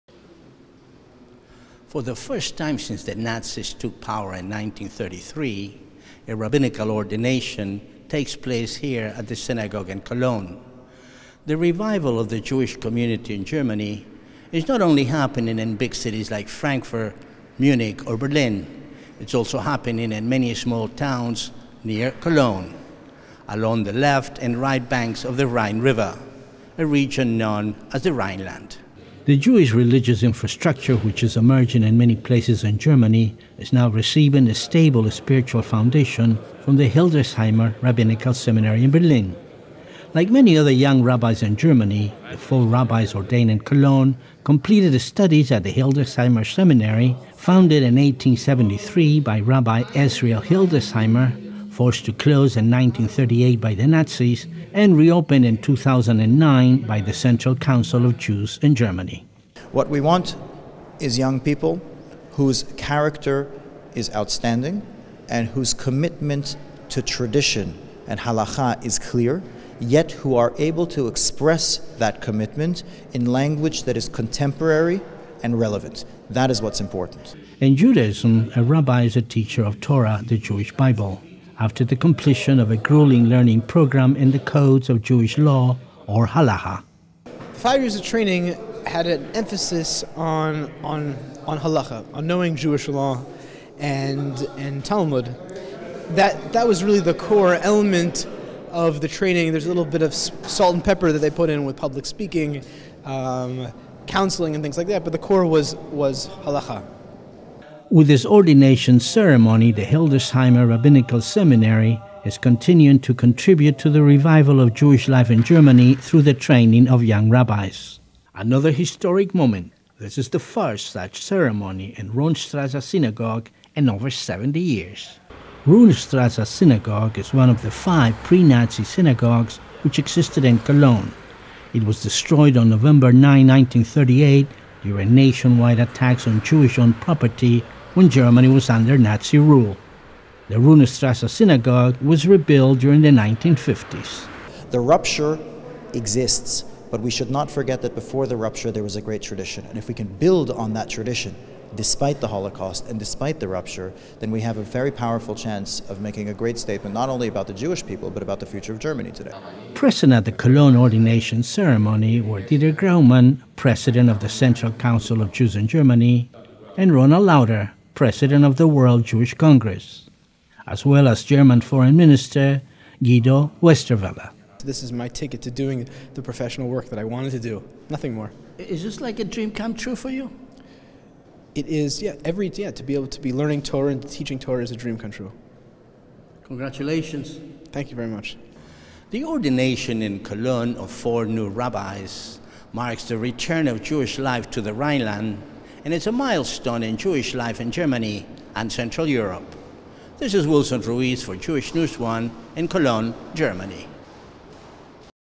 Historic Rabbis Ordination in Cologne Germany
For the first time since the Nazis took power in 1933, a rabbinical ordination takes place here at the synagogue in Cologne. The revival of the Jewish community in Germany is not only happening in big cities like Frankfurt, Berlin or Munich it’s also happening in many small towns around Cologne, along the left and right banks of the Rhine River, a region known as the Rhineland.